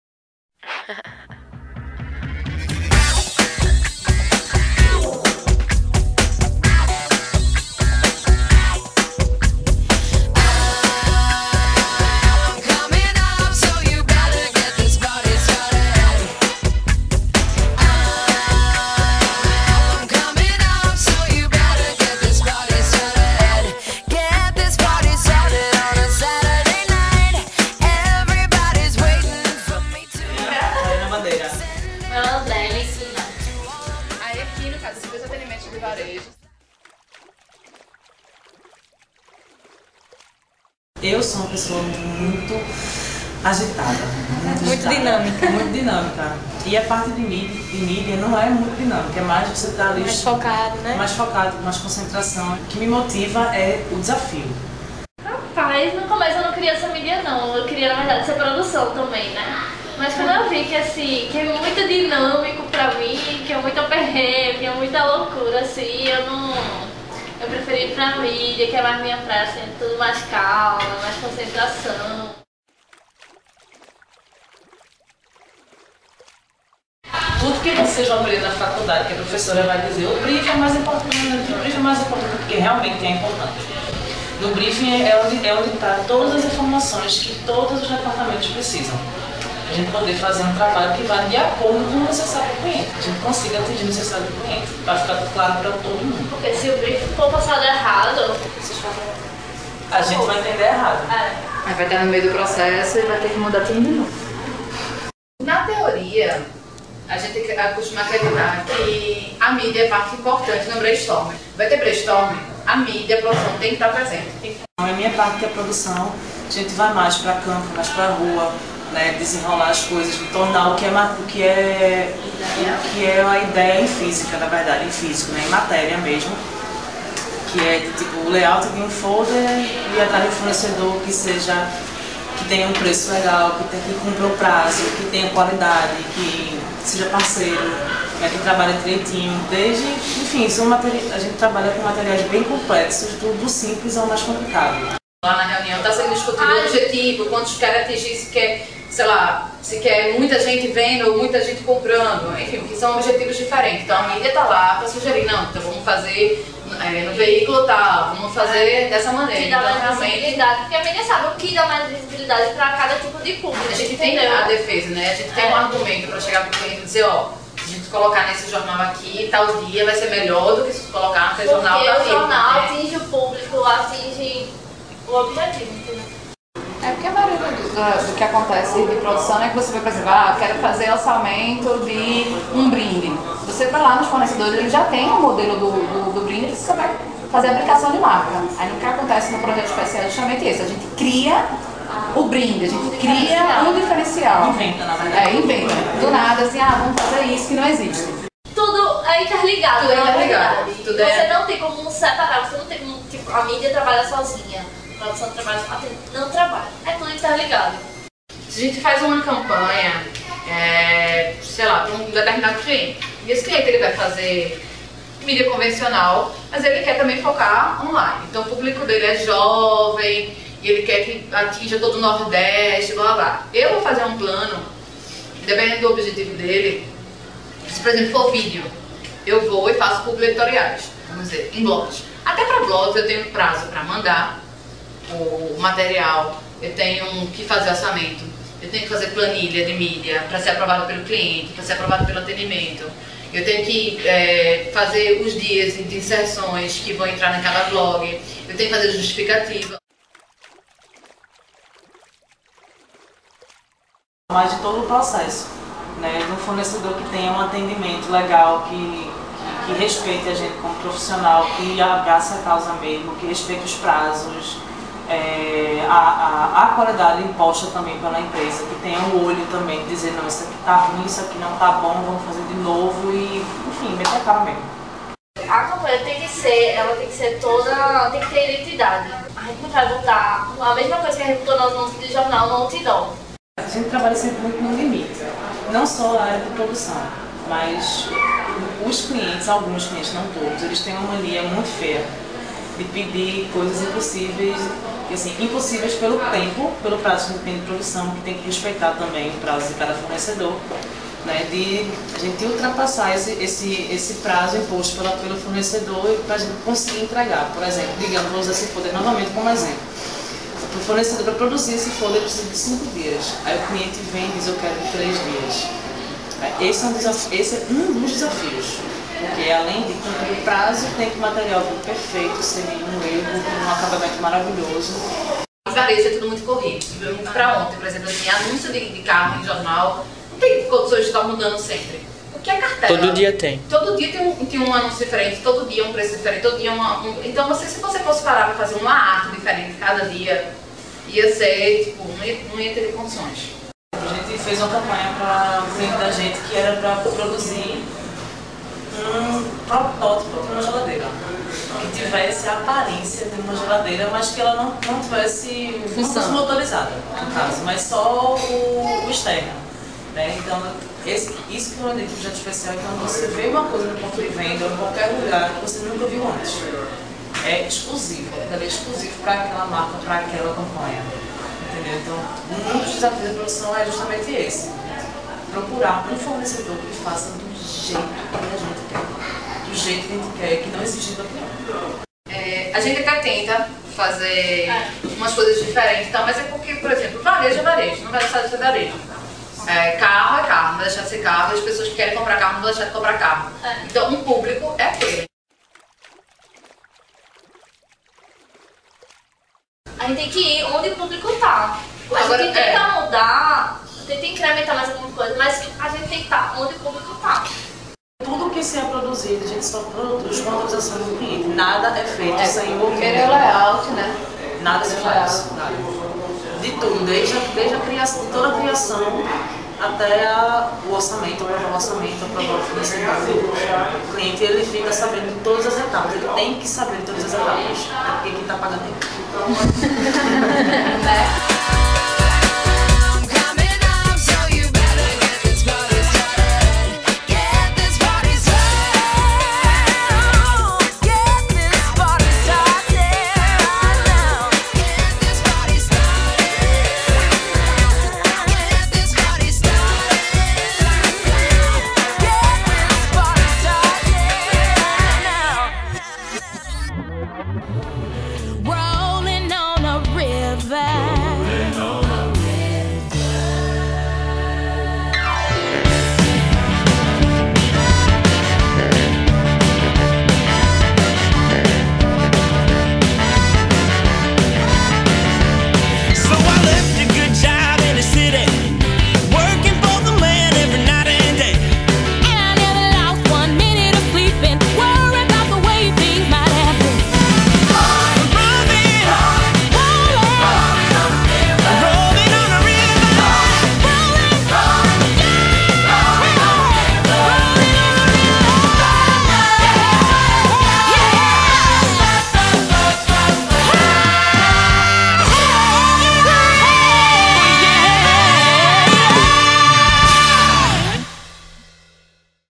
Visita à Agência Hagua
Trabalho realizado para a cadeira de Introdução à Publicidade e Propaganda ministrada pelo professor Sergio Mendonça.
Alunos do 1º período-manhã de Publicidade da Faculdade FAMA - Escola Superior de Marketing.

Grupo de Mídia: Gabriela Cunha, Juliana Vasconcelos, Kelly Oliveira, Mariana Menezes e Robson Souza.
Grupo de Produção: Julliany Didier, Matheus Menezes, Tafaiane Espindola e Olimpia Rodrigues.

Visita Técnica à Agência Hagua, em Recife.

Agradecimentos: Agência Hagua, Carol Braga, Mariana Bandeira e Andrelly Pina.